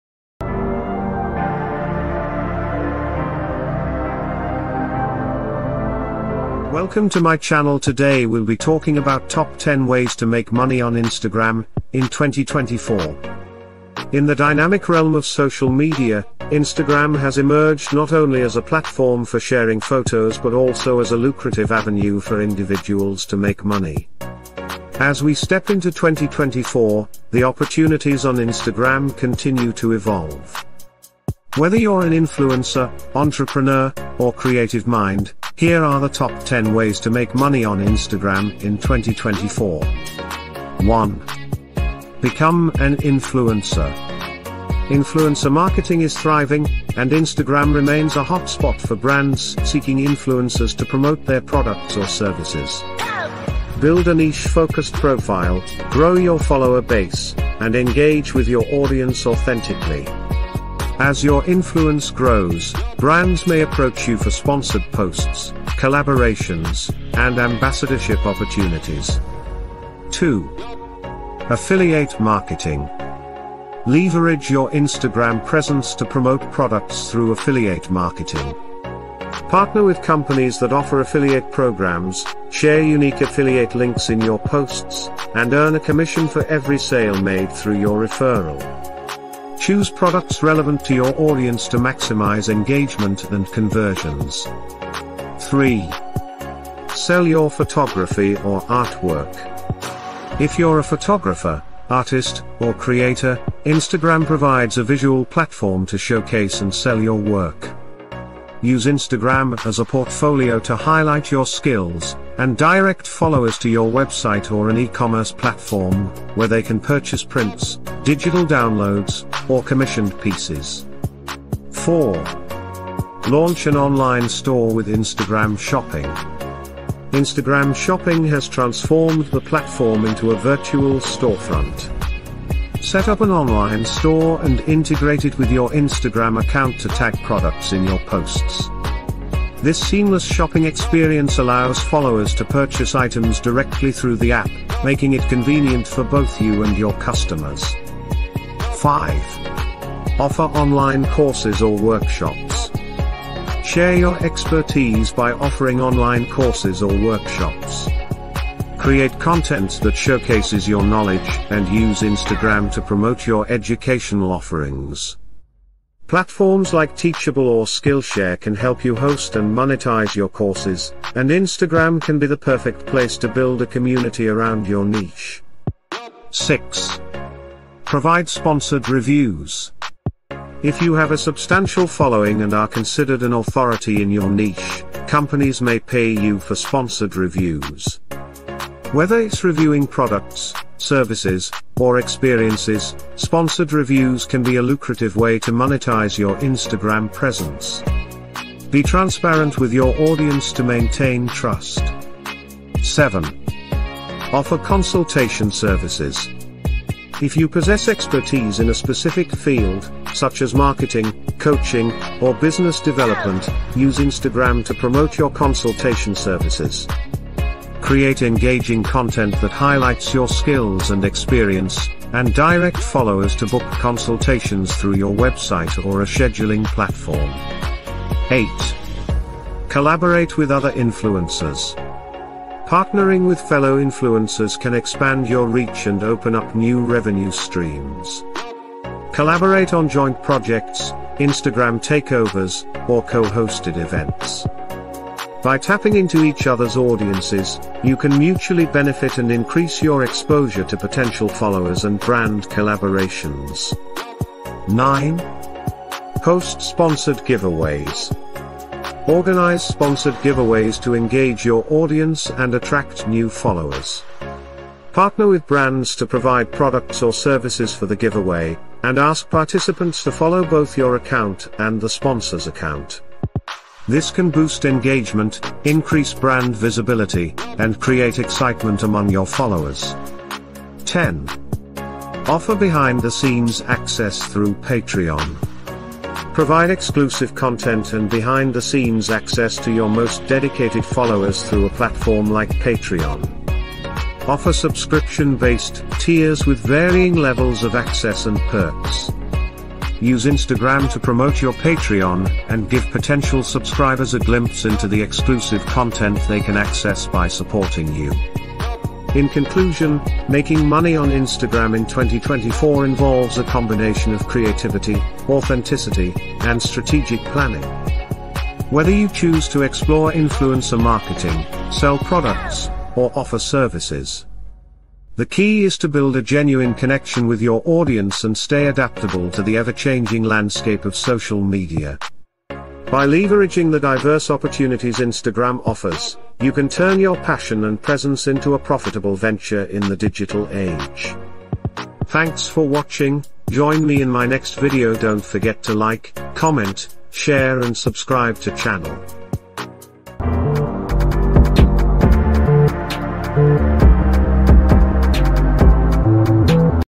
Top 10 ways to make money on Instagram 2024
Top 10 Ways to Monetize Your Instagram in 2024 - The Online Dollar King

Discover the most effective strategies for monetizing Instagram in 2024 with The Online Dollar King's latest video. Dive into our comprehensive guide covering the top 10 methods to turn your Instagram account into a lucrative venture. Learn how to leverage Instagram shopping, offer online courses, provide sponsored reviews, and much more. Whether you're a seasoned influencer or just starting out, our video offers valuable insights into maximizing your earnings on Instagram. Stay ahead in the dynamic world of social media and transform your passion into profit. Don't miss out on these cutting-edge tips and tricks - watch now and start growing your Instagram income today!

 From sponsored posts to marketing strategies, these tips will help you grow your Instagram account and make money!

Whether you're a beginner or a experienced Instagram user, these tips will have you earning money in no time! By following these strategies, you'll be on your way to a successful online career!